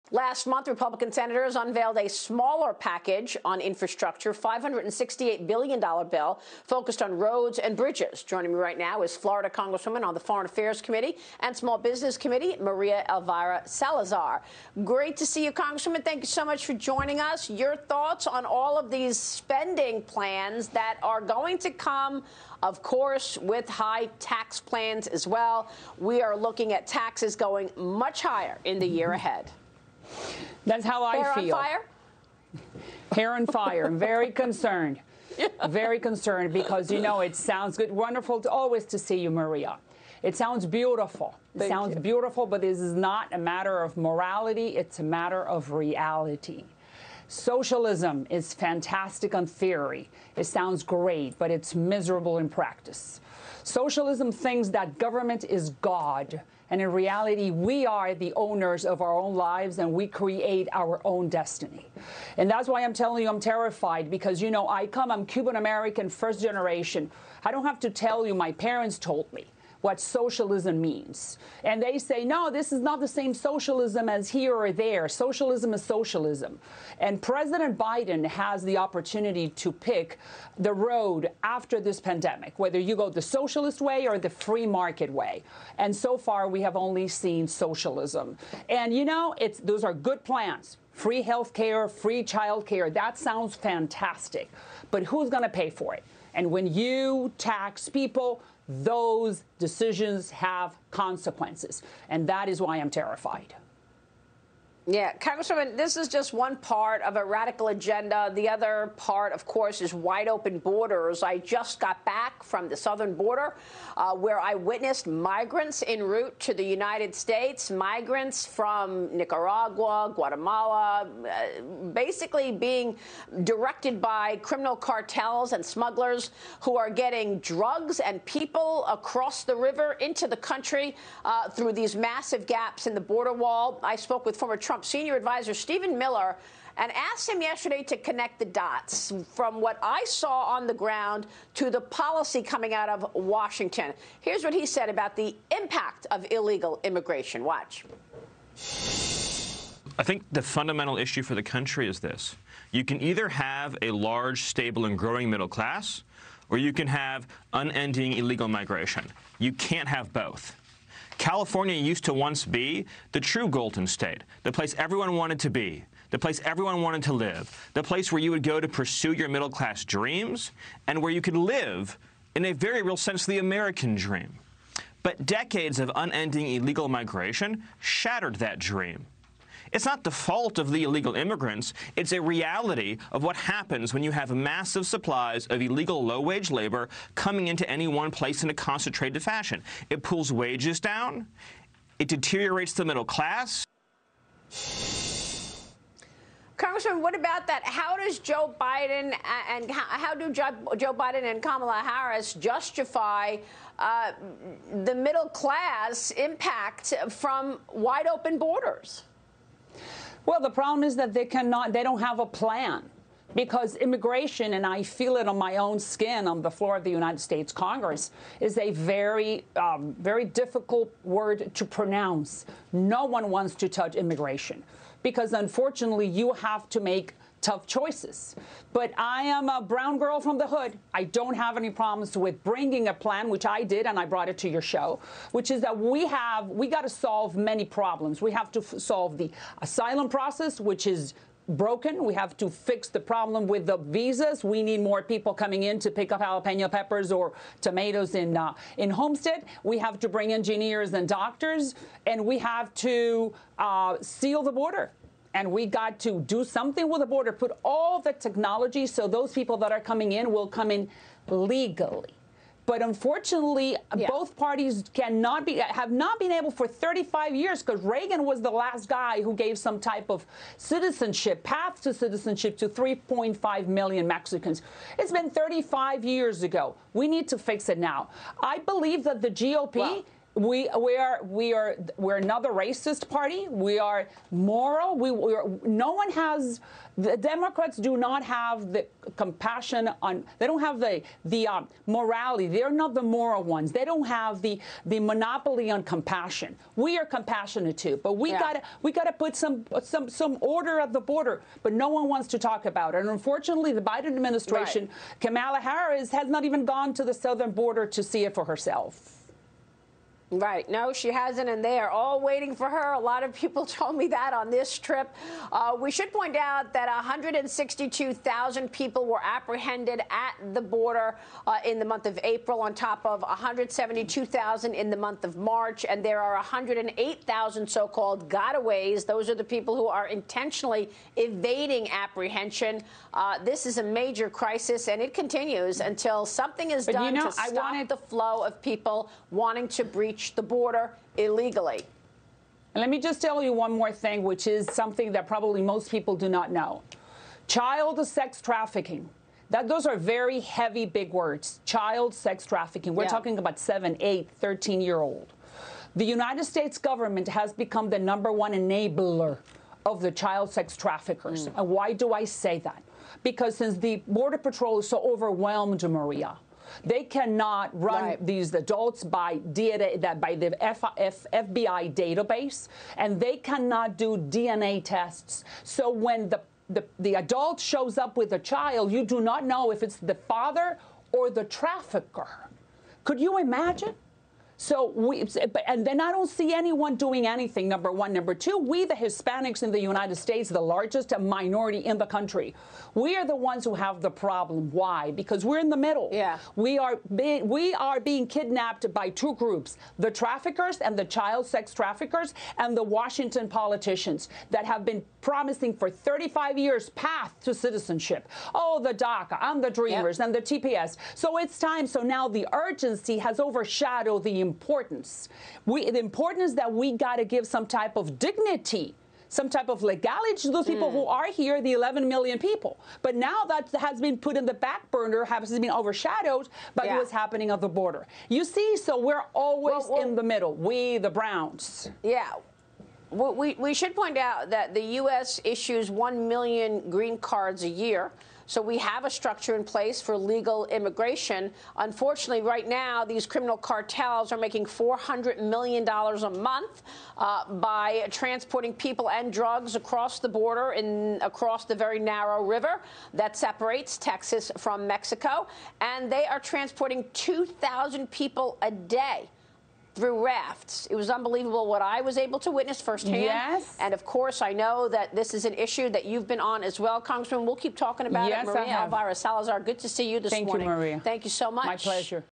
Biden, Harris have no border plan: Florida lawmaker
Rep. Maria Elvira Salazar, R-Fla., shares her thoughts on spending and taxes under President Biden and the immigration crisis facing the U.S.